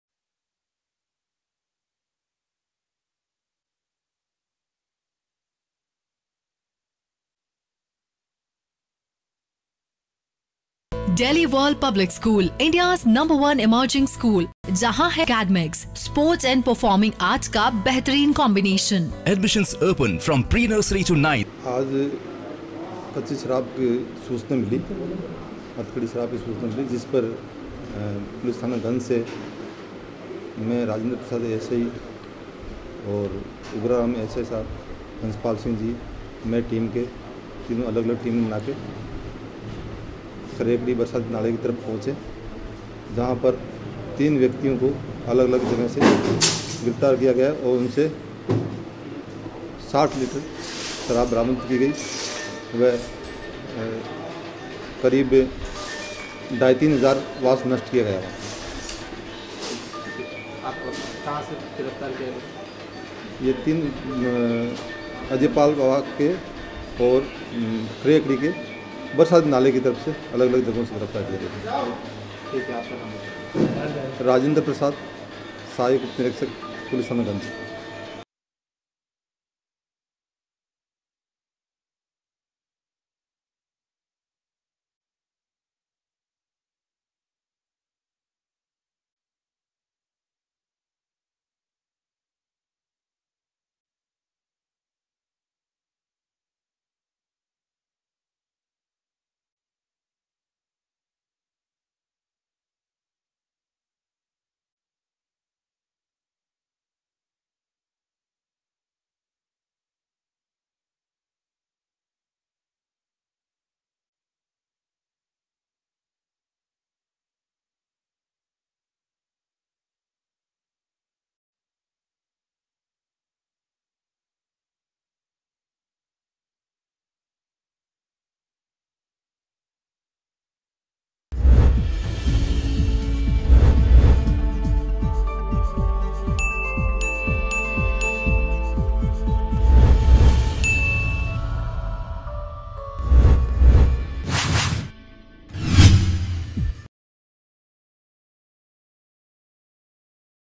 AJMER NEWS | गंज थाना पुलिस ने हथकड़ शराब के साथ तीन आरोपियों को किया गिरफ्तार | MTTV INDIA
गंज थाना पुलिस ने हथकड़ शराब के साथ तीन आरोपियों को किया गिरफ्तार,60 लीटर अवैध हथकड़ शराब और 2500 लीटर वॉश किया नष्ट,खरखेड़ी और अजयसर गाँव में गंज थाना पुलिस की कार्रवाई